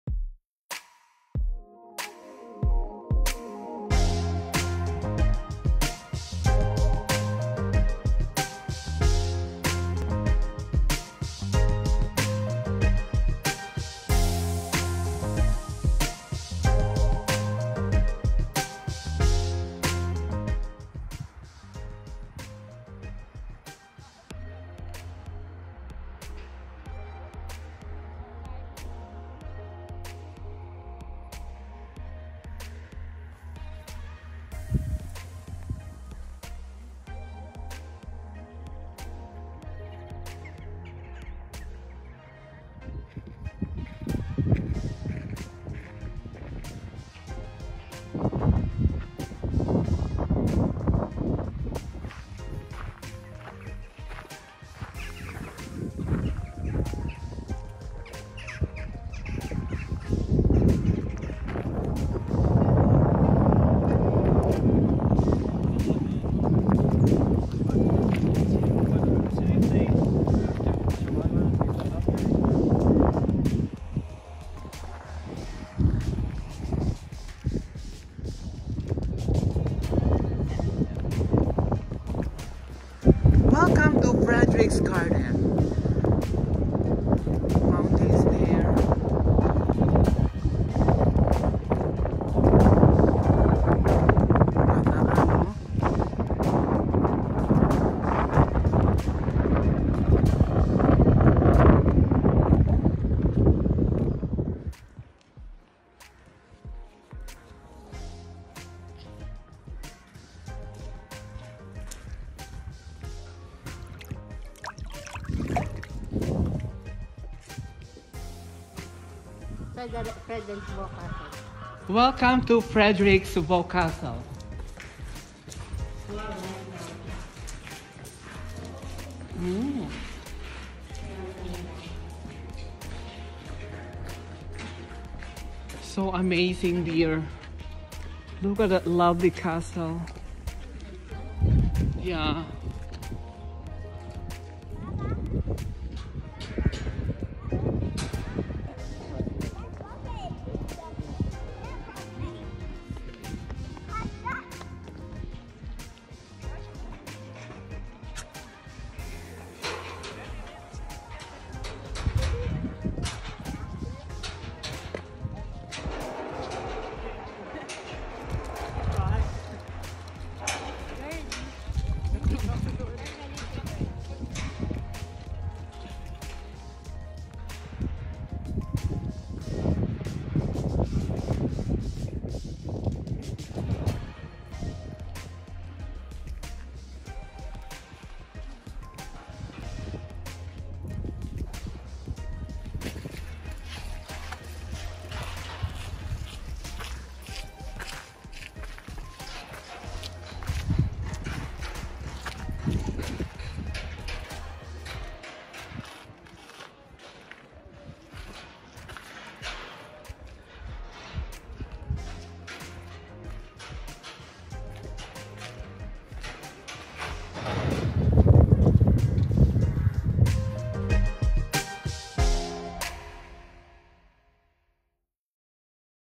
FREDERIKSBORG CASTLE, DENMARK!
Frederiksborg Castle is a palatial complex in Hillerød, Denmark. It was built as a royal residence for King Christian IV of Denmark-Norway in the early 17th century, replacing an older castle acquired by Frederick II and becoming the largest Renaissance residence in Scandinavia.